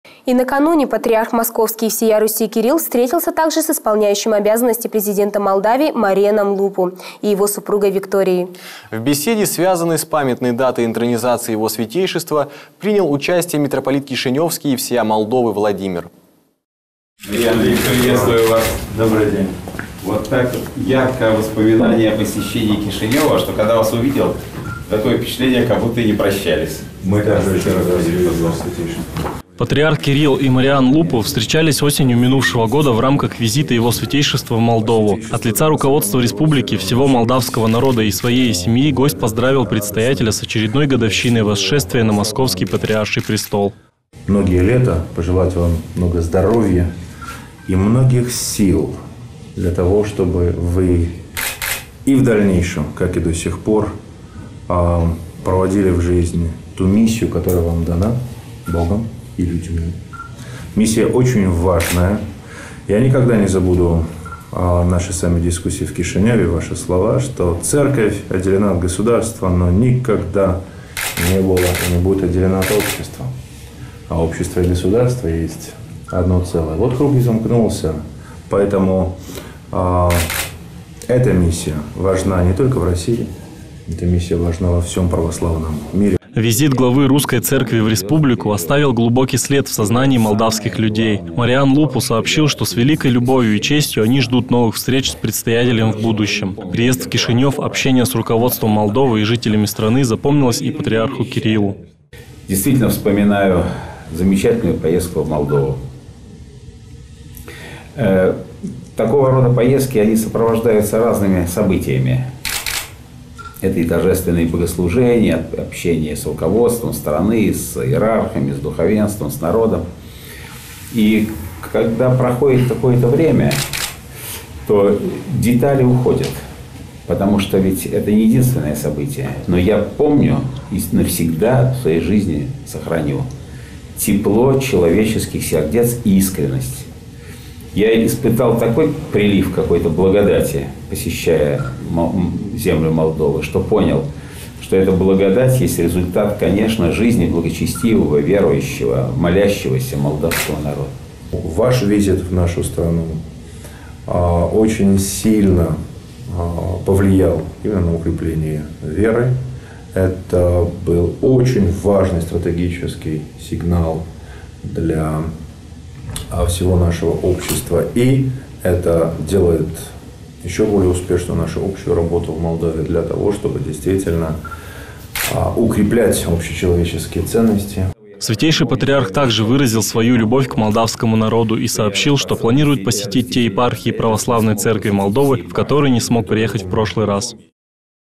Патриарх Кирилл встретился делегацией из Молдавии
Состоялась встреча Святейшего Патриарха Кирилла с председателем Правительства России Владимиром Владимировичем Путиным.